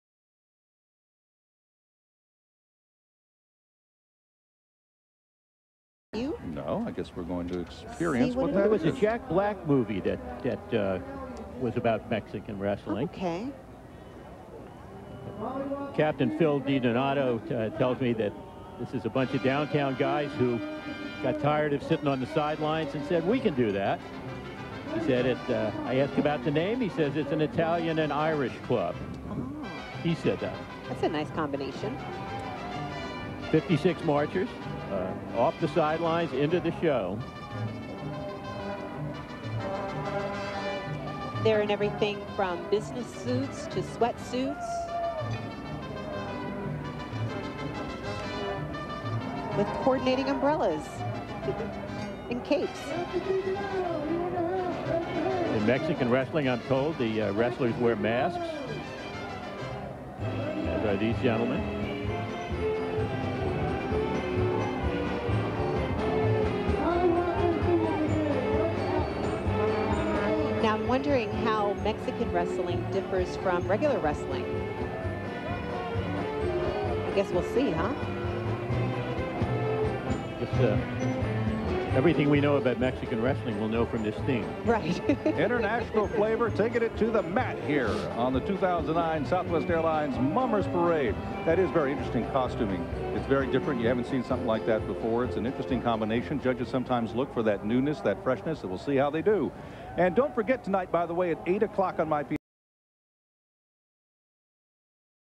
Mollywoppers N.Y.B. - Meximum Wrestling - 2009
Mollywoppers N.Y.B. Est. 2008. This was our first year in the parade. I finally got around to digitizing this VHS tape and putting this up. Enjoy!